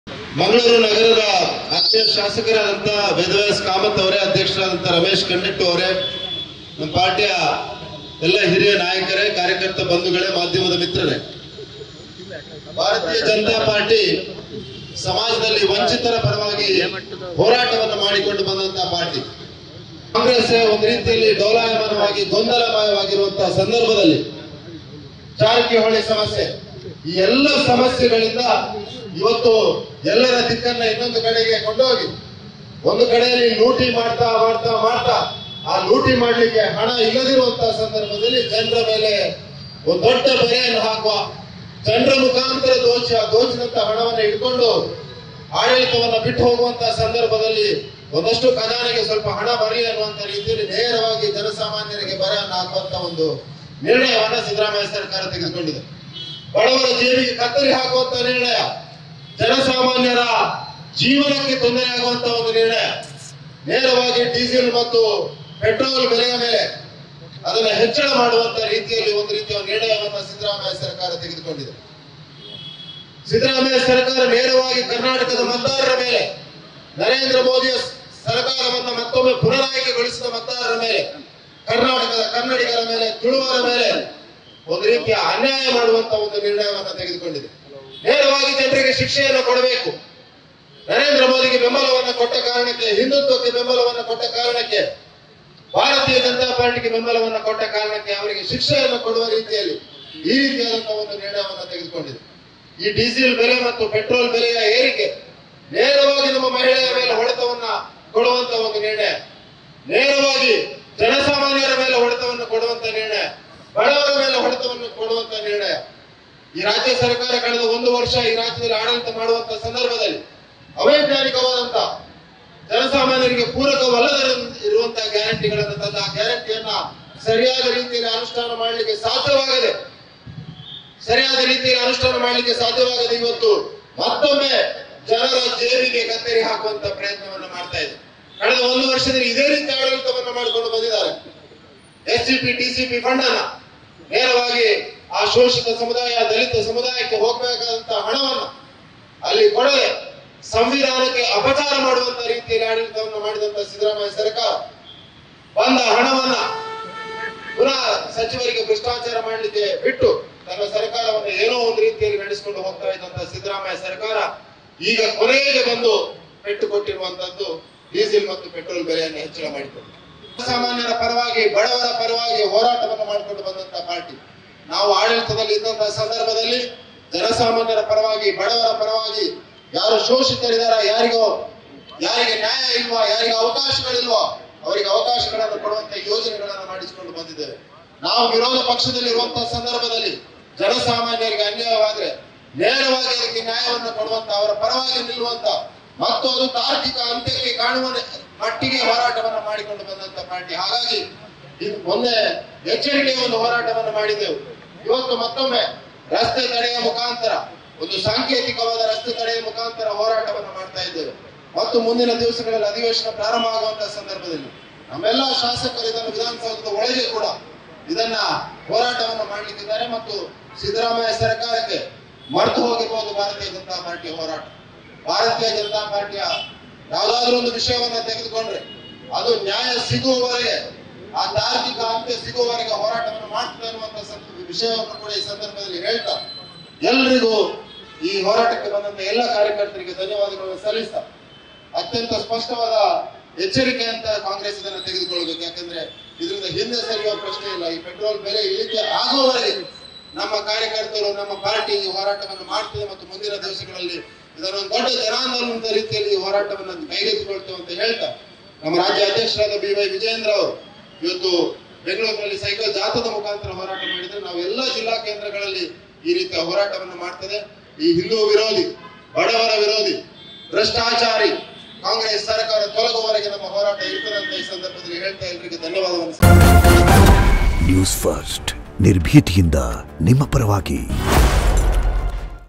Brijesh Chowta : ಜನ Modi & ಹಿಂದುತ್ವಕ್ಕೆ ಬೆಂಬಲ ಕೊಟ್ಟಿದ್ದಕ್ಕೆ ಜನರಿಗೆ Congress ಶಿಕ್ಷೆ ಕೊಡ್ತಿದೆ !
Brijesh Chowta : ಜನ PM Narendra Modi & ಹಿಂದುತ್ವಕ್ಕೆ ಬೆಂಬಲ ಕೊಟ್ಟಿದ್ದಕ್ಕೆ ಜನರಿಗೆ Congress ಶಿಕ್ಷೆ ಕೊಡ್ತಿದೆ ! | Mysuru | @newsfirstkannada